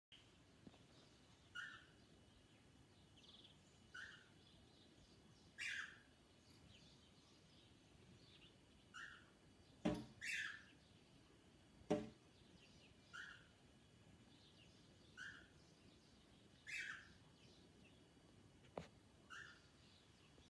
An Almost Peaceful Day
There was 1 bird there but then his annoying cousin came along. (No Birds were harmed in the making of this video)